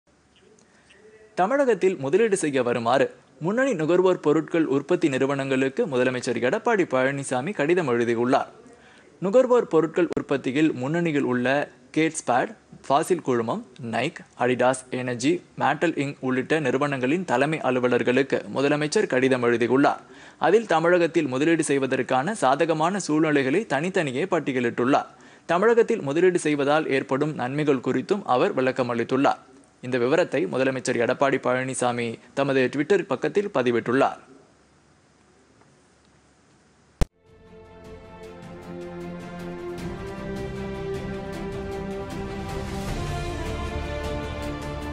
முன்னணி நிறுவனங்களுக்கு முதல்வர் அழைப்புPodhigaiTamilNews
தமிழகத்தில் முதலீடு செய்ய வருமாறு முன்னணி நுகர்வோர் உற்பத்திப் பொருட்கள் நிறுவனங்களுக்கு முதலமைச்சர் எடப்பாடி பழனிசாமி அழைப்பு.